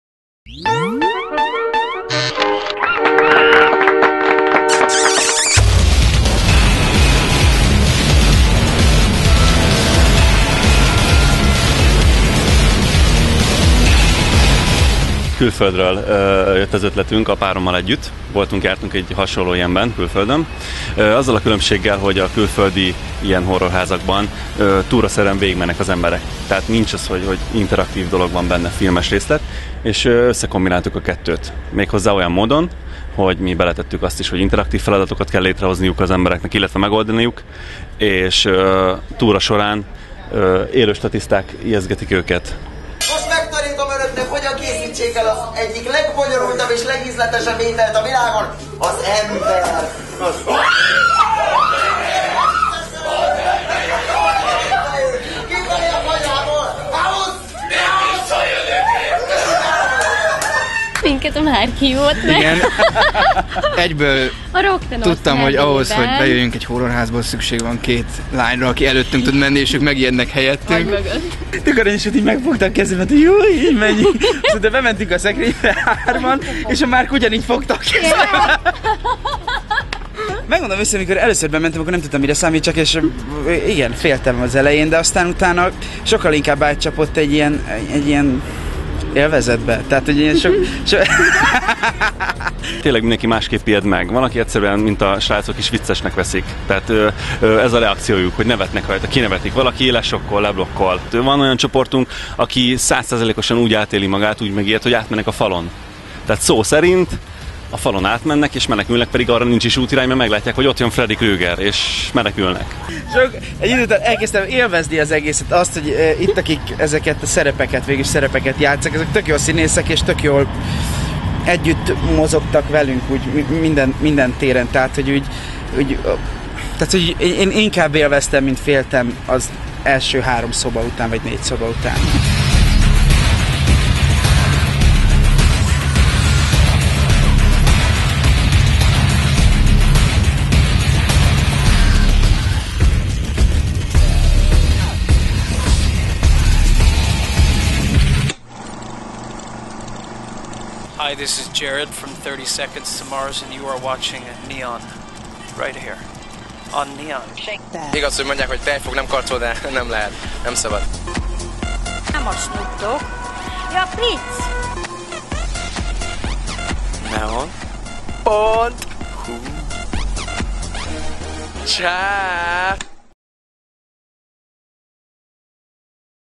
Rocktenors az Underground Fearben
Horrorlabirintusban az X-Faktoros csapatok